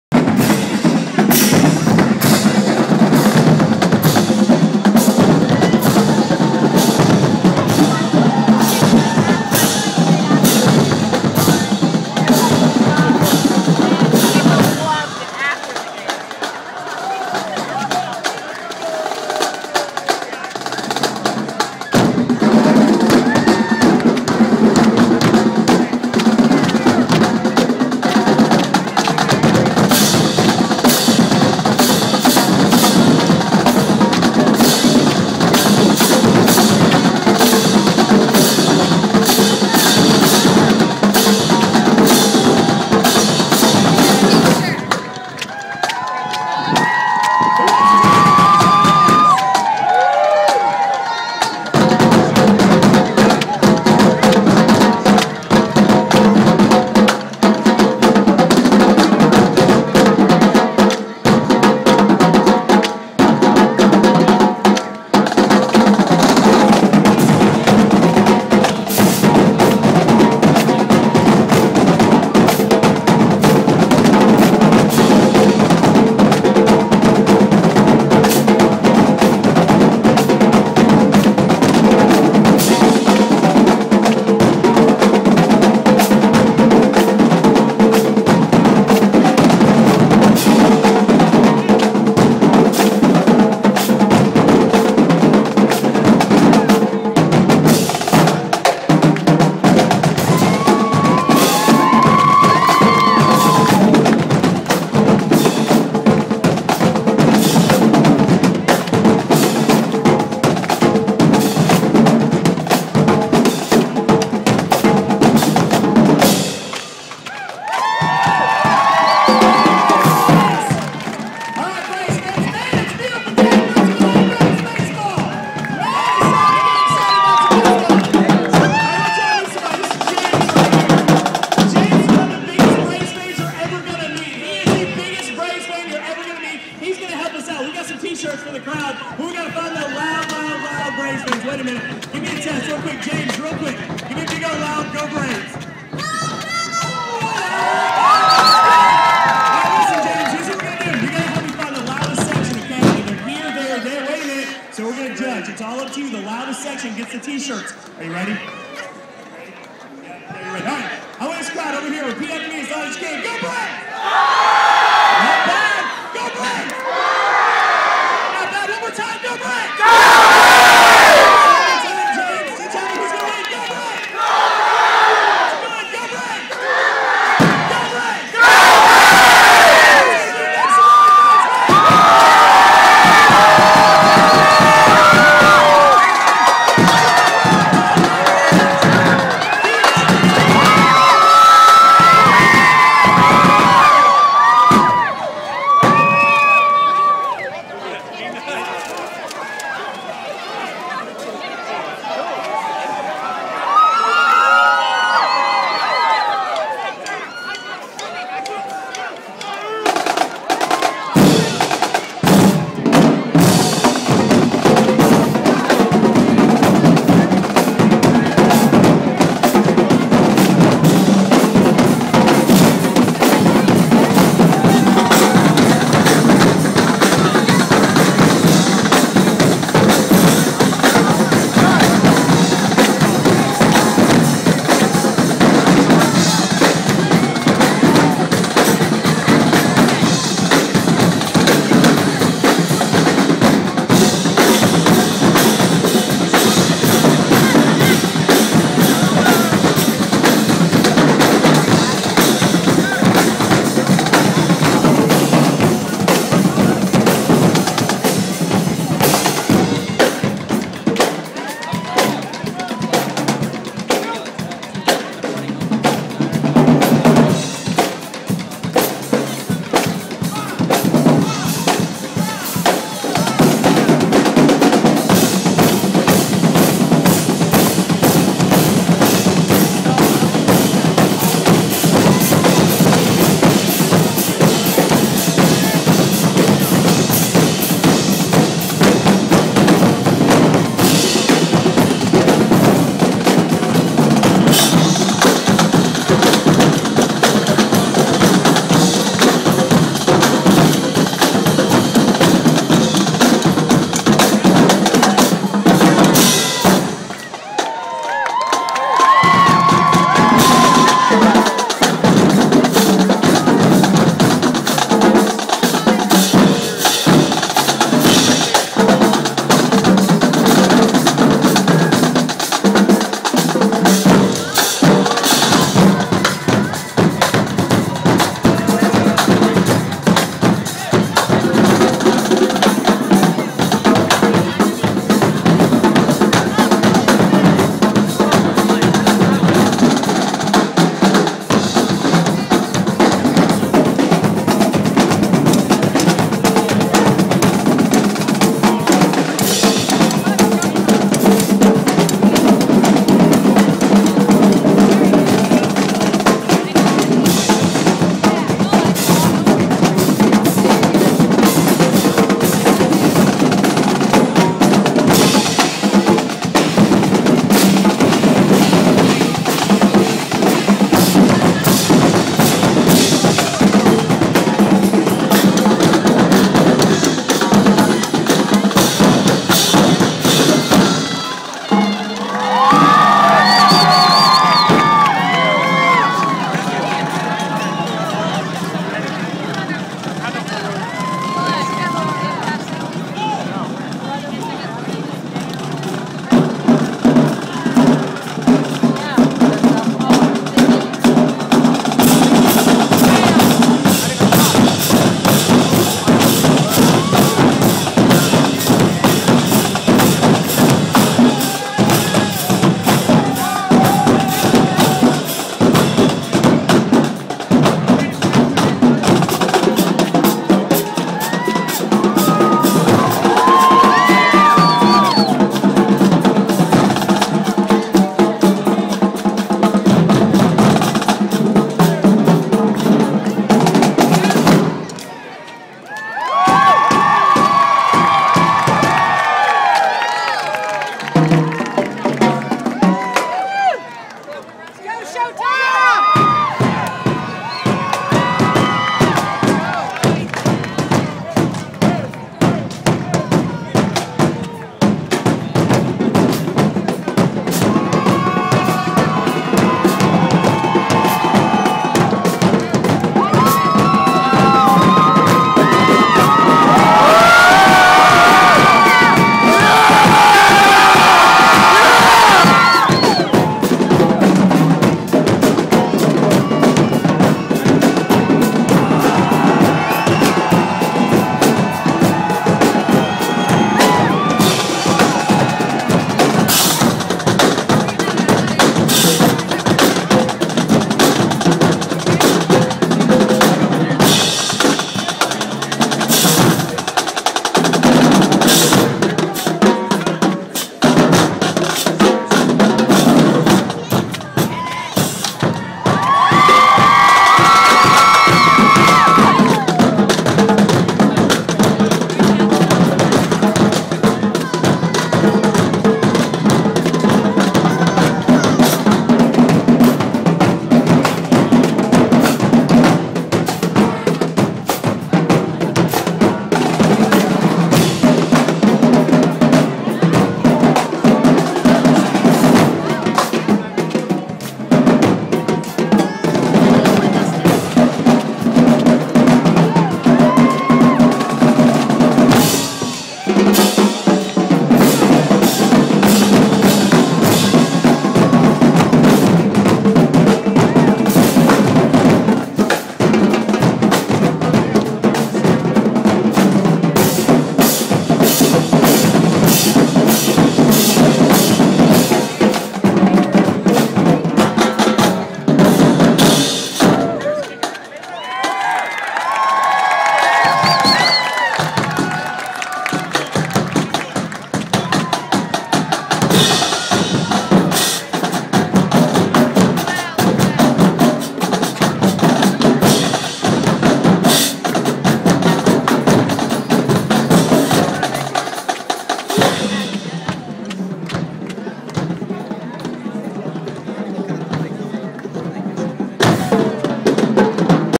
Braves Drummers at the Chop Rally 7-18-09
The Atlanta Braves Drumline performing pre game during the Chop Rally